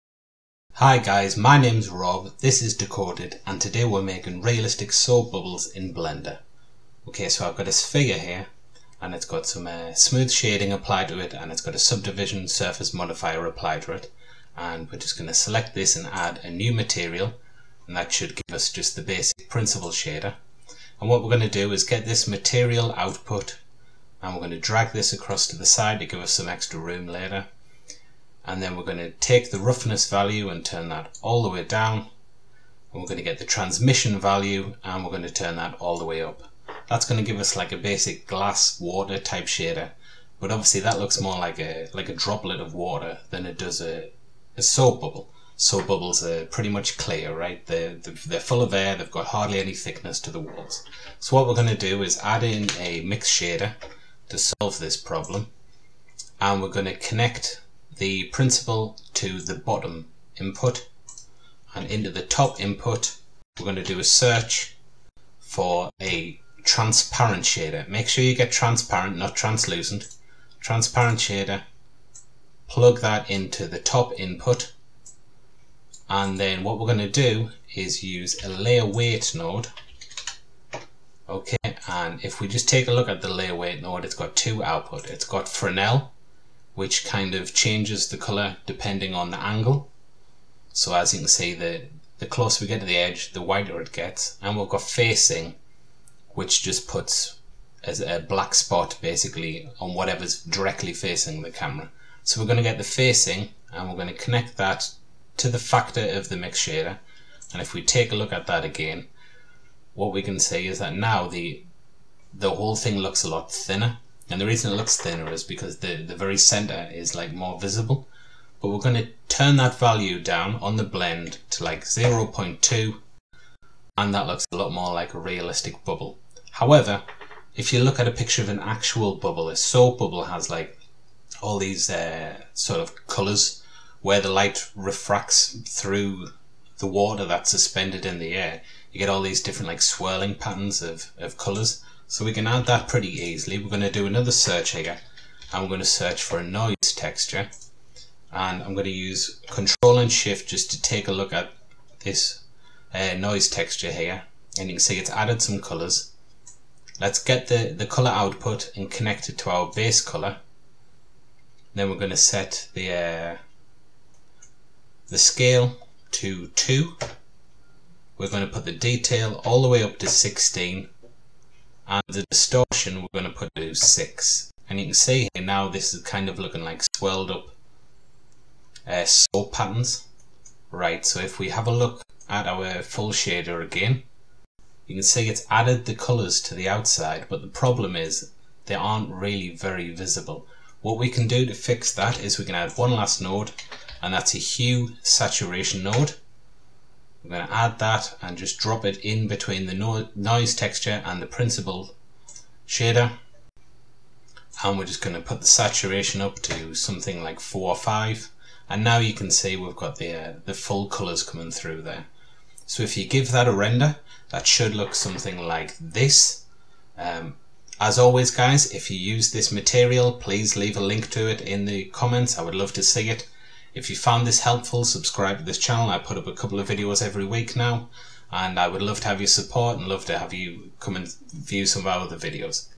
Blender Tutorial: Realistic Bubbles In Four Minutes
Learn how to make a super-quick soap bubble material in Blender. Not only is this shader photo-realistic, but it takes just minutes to set up and it's completely procedural.